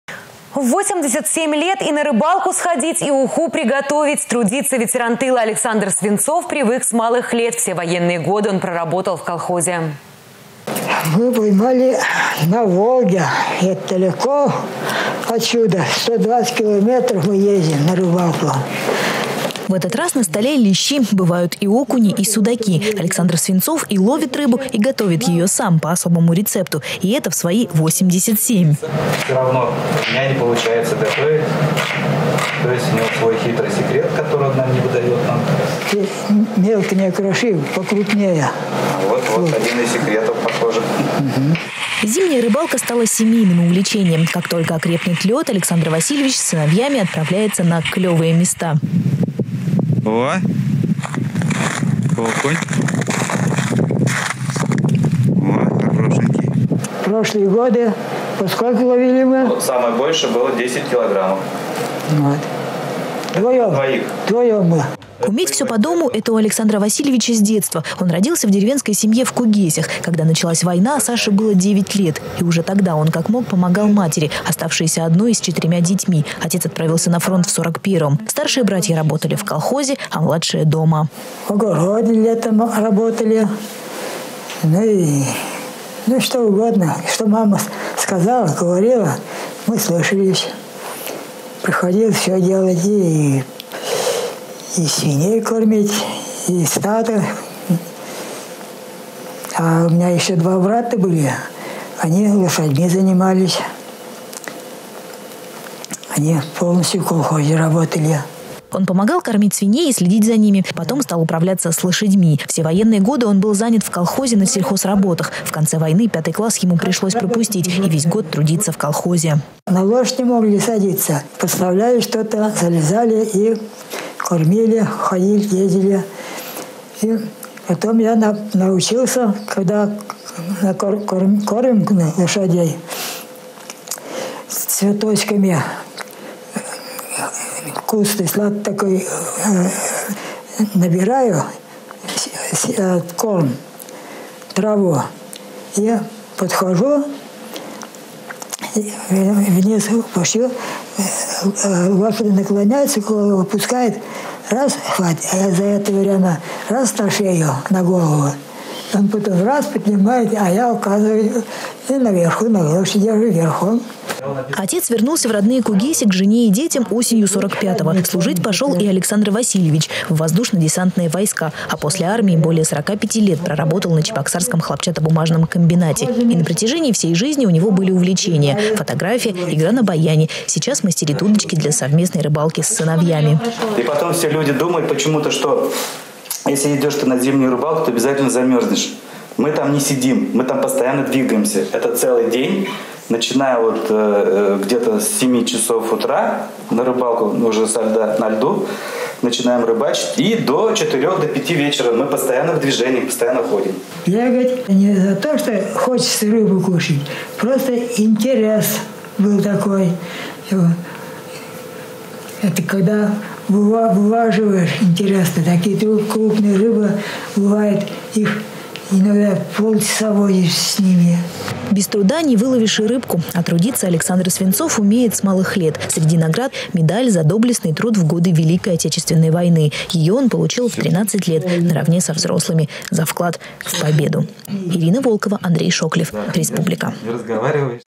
В 87 лет и на рыбалку сходить, и уху приготовить. В гостях у ветерана тыла Александра Свинцова
В 87 лет и на рыбалку сходить, и уху приготовить. Трудиться ветеран тыла Александр Свинцов привык с малых лет. Все военные годы он проработал в колхозе.